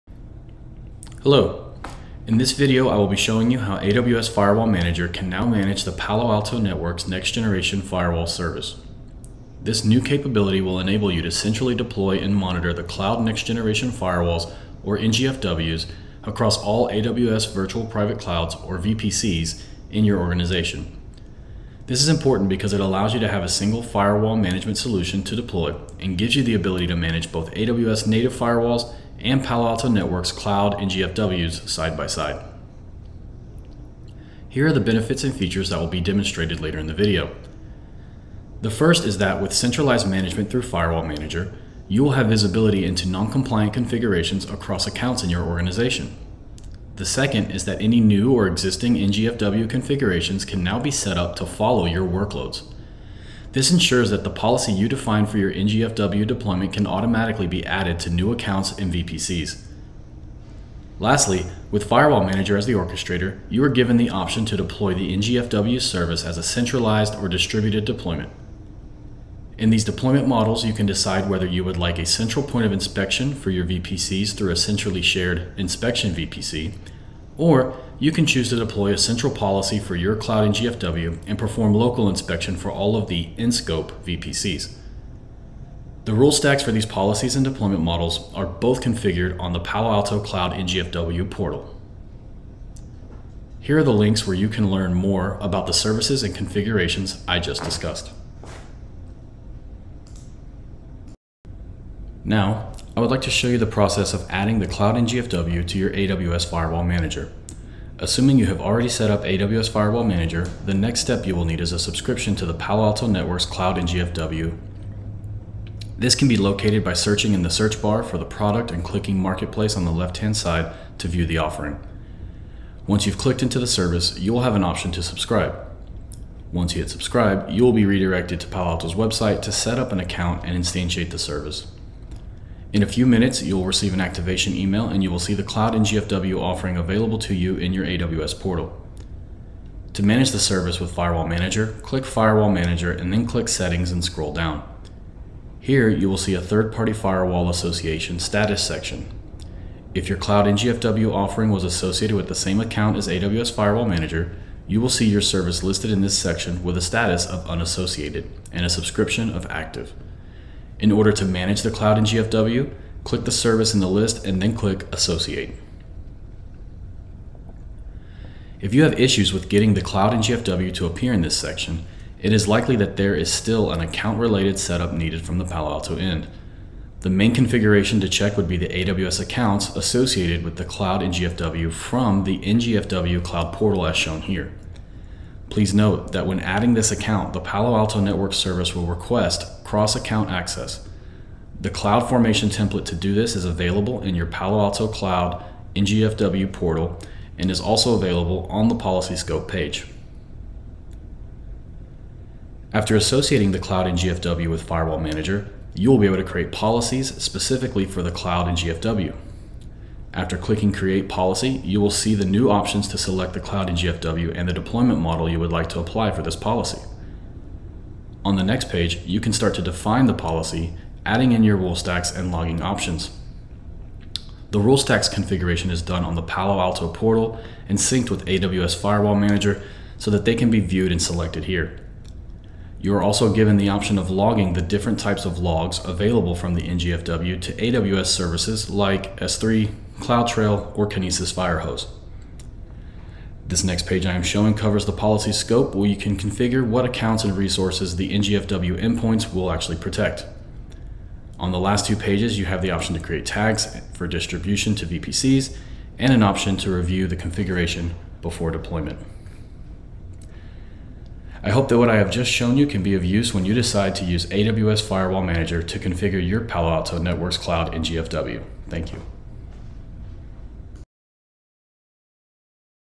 AWS Firewall Manager support for Palo Alto Networks Cloud NGFW | Amazon Web Services
AWS Firewall Manager enables you to centrally deploy and monitor Palo Alto Networks Cloud Next Generation Firewalls (NGFWs) across all AWS virtual private clouds (VPCs) in your AWS organization. With this release, customers will now have a single firewall management solution to deploy and manage both AWS native firewalls and Palo Alto Networks Cloud NGFWs.

Important Links:
•AWF Firewall Manager

More AWS events videos ABOUT